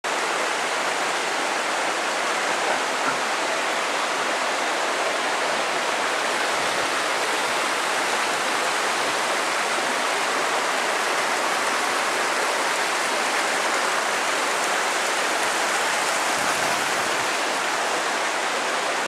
Walton Glen: "The Eye of The Needle"
The craziest part of the entire Walton Glen Protected area, The Eye of the Needle measures approximatly 3m wide x 90m high.